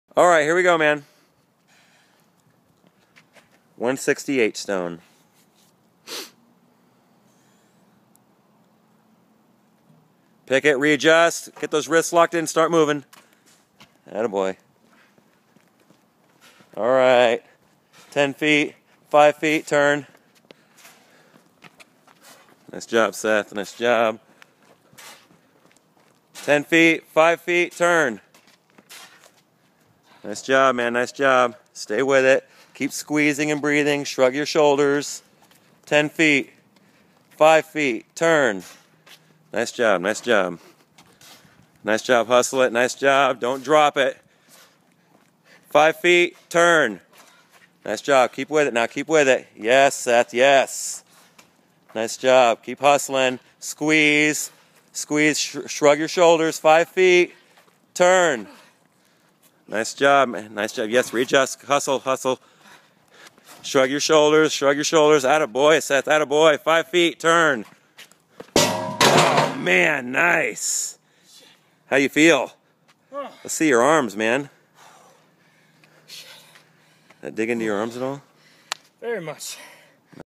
Seth Brannan H-Stone
Beastmode Training 160lbs 7.5" H-stone x 300ft turns every 50ft